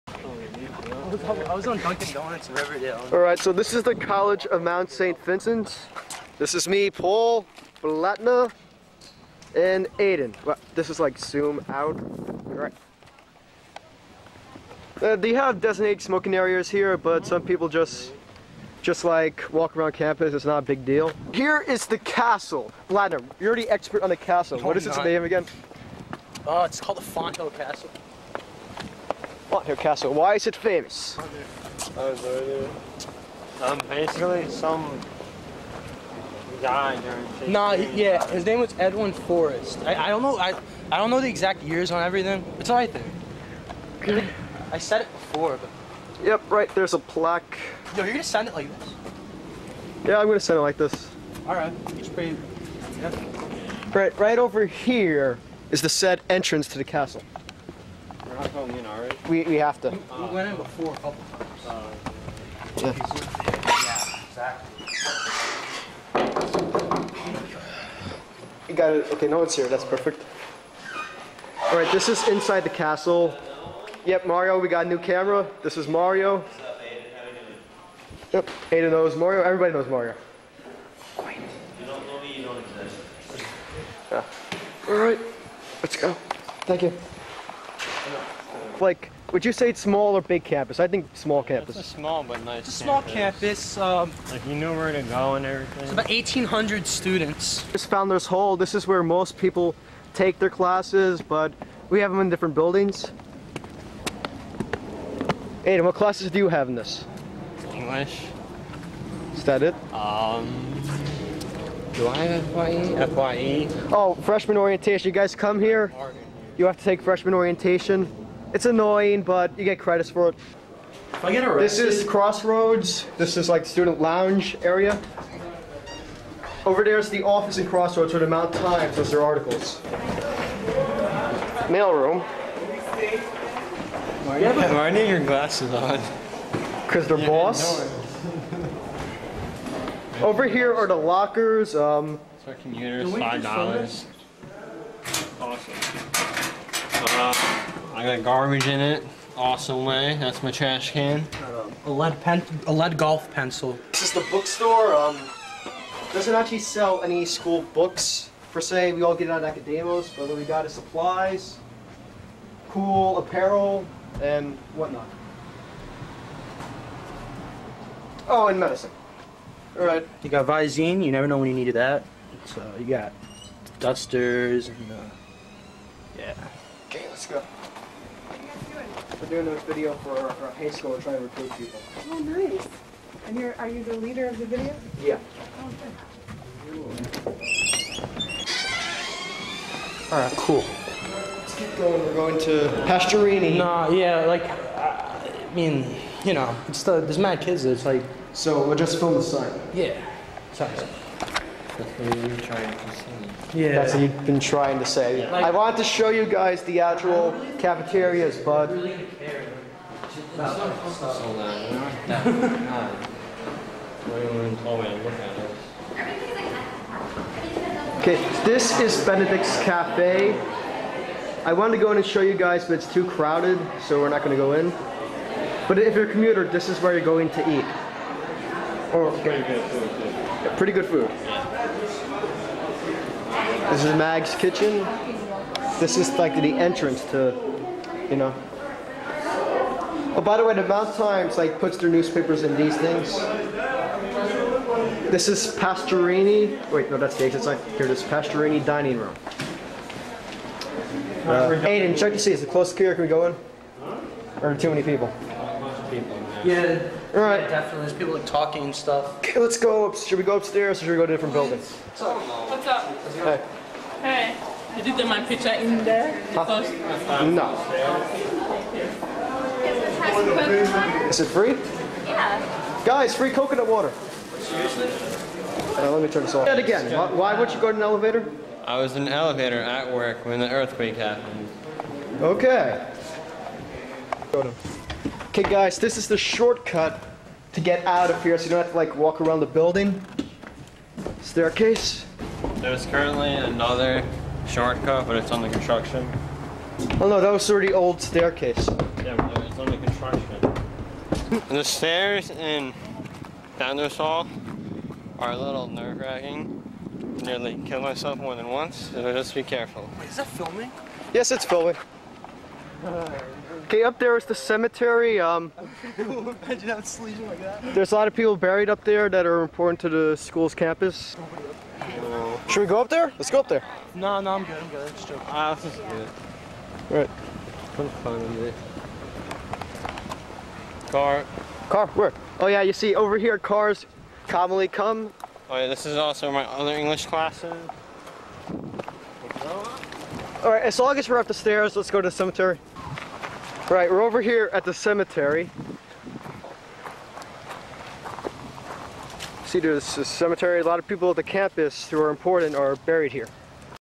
The College of Mt. Saint Vincent tries to recruit the Salesian Class of 2012!
Featureing aidan mcSweeny, and paul mcGovern The Irish Wolf Hound Of America. Also,.. anyone unfortunate enough to come in path of the camera.